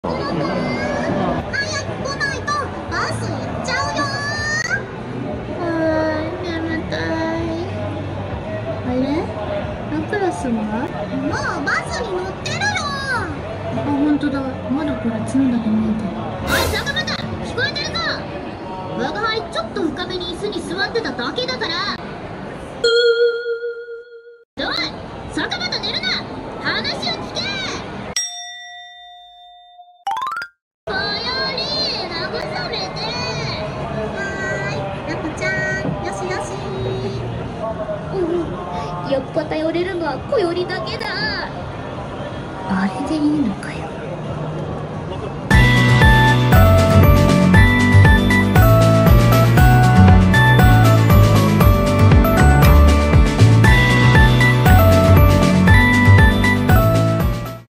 HoloX on holoEXPO bus | hololive Fan Animation
Source: I Underestimated the World's BIGGEST hololive Event

holoX member
★ラプラス・ダークネス / La+ Darkness
★博衣こより / Hakui Koyori
★沙花叉クロヱ / Sakamata Chloe